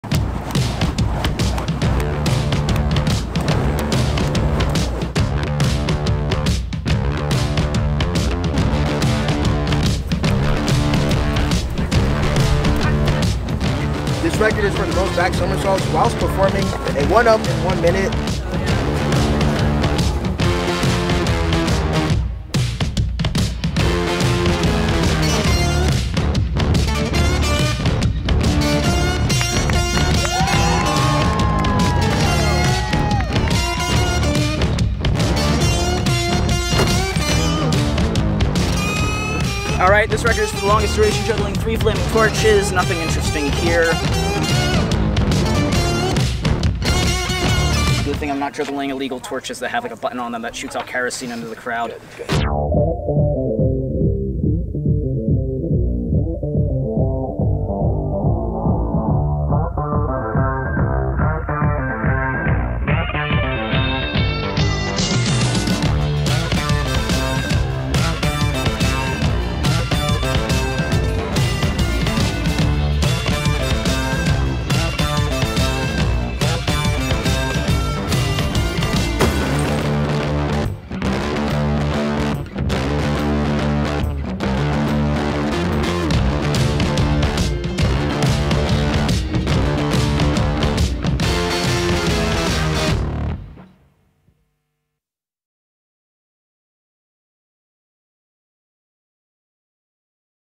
Three World Records Broken at Eckerd College
Congrats to Eckerd College junior Aidan Webster! He broke three Guinness World Records for juggling—then headed back to the marine science lab. Read the full story

Video: Penh Alicandro '22